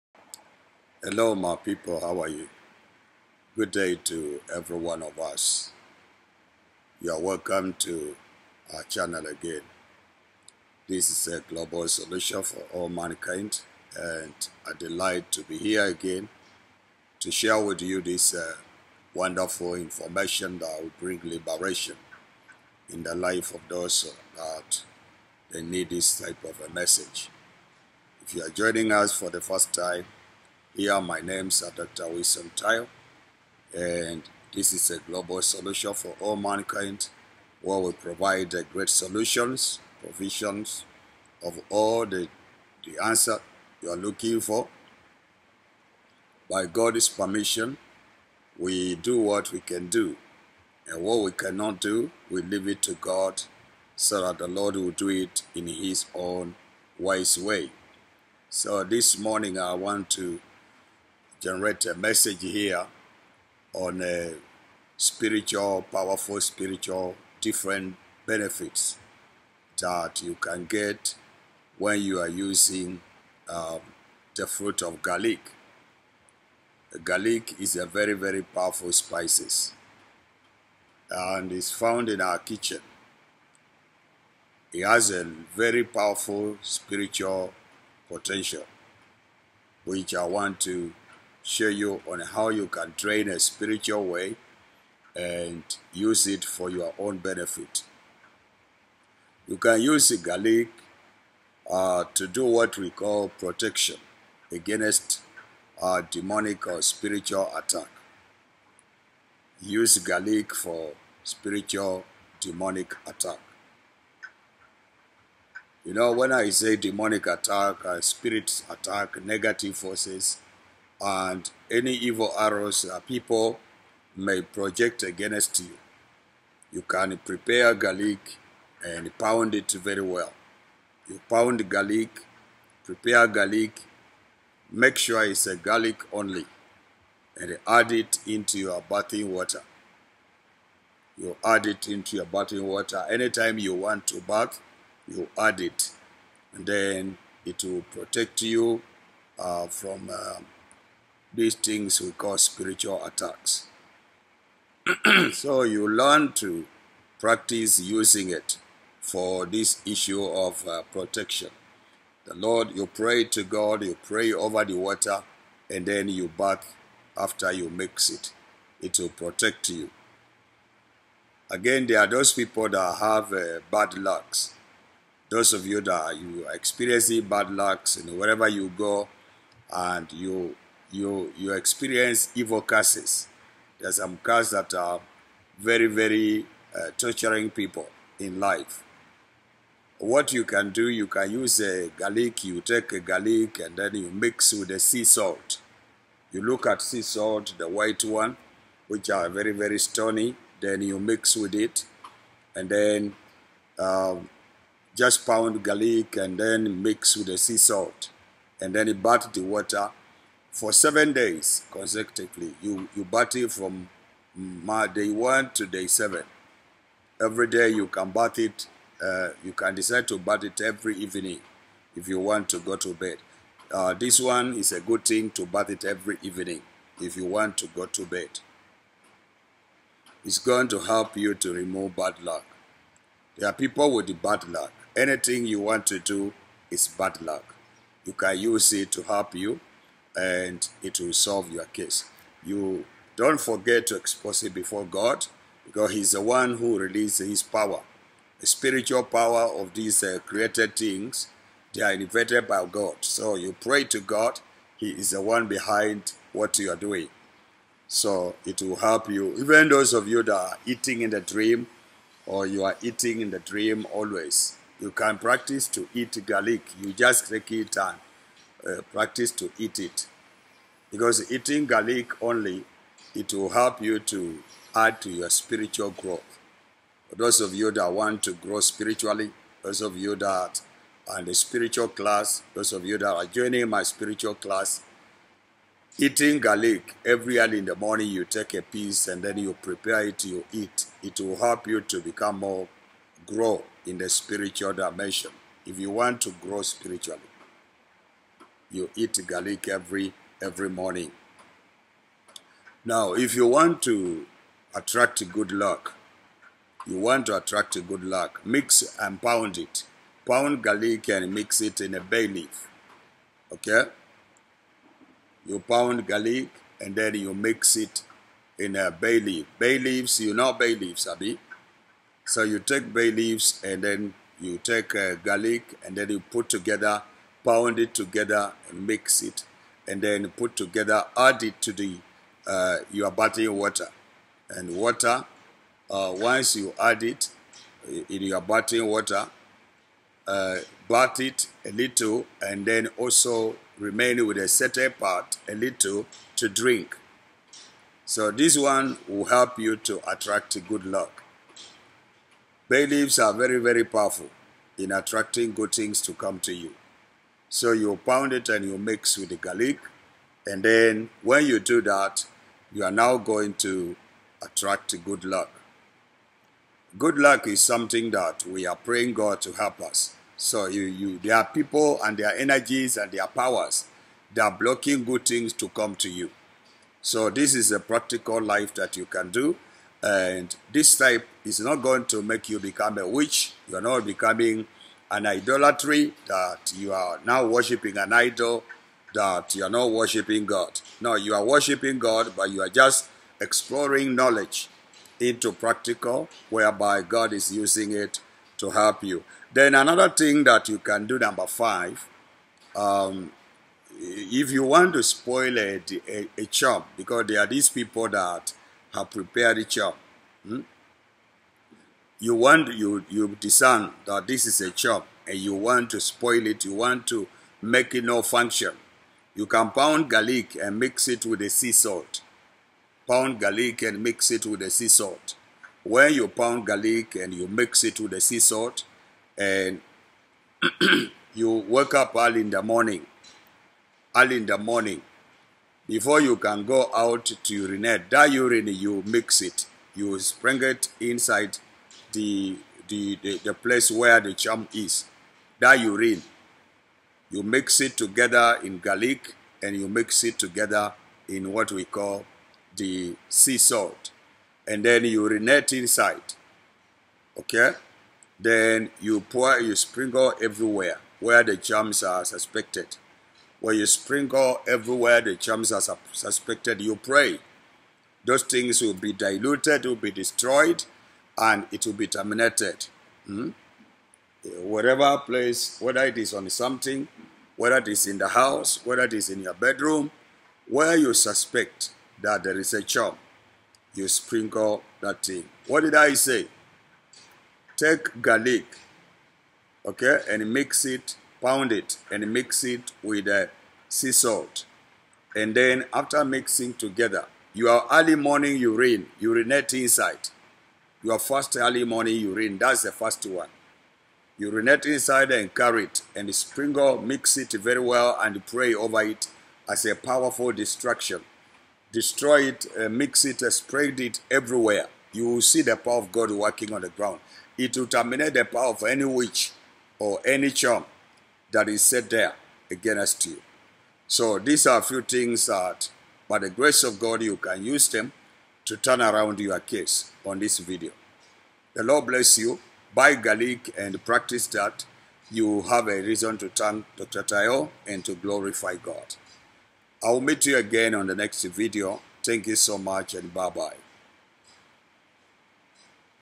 MIX GARLIC AND BAY LEAVES TO PULL GOOD LUCK AND MORE
Destroying charm with garlic mix with sea salt and early urine sprinkle the place we have charm